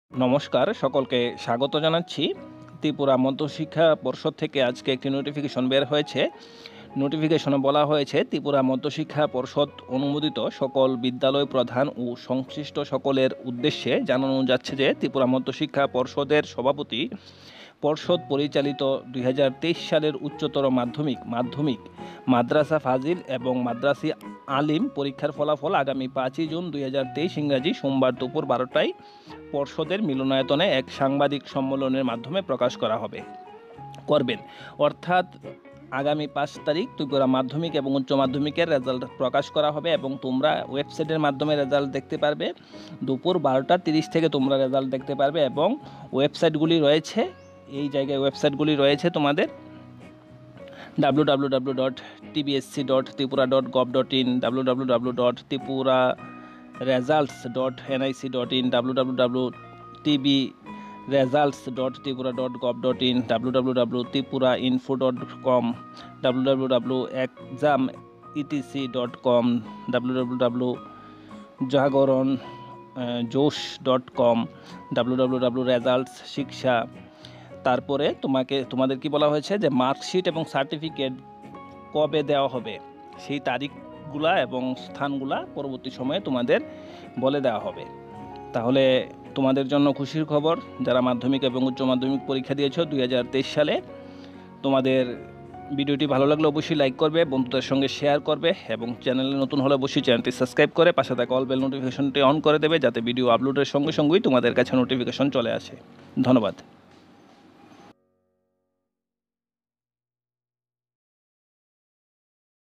ত্রিপুরা মাধ্যমিক এবং উচ্চ মাধ্যমিক পরীক্ষা-২০২৩ | ফলাফল প্রকাশ করা হবে ৫-ই জুন।
tripura_hs_resuts_2₩23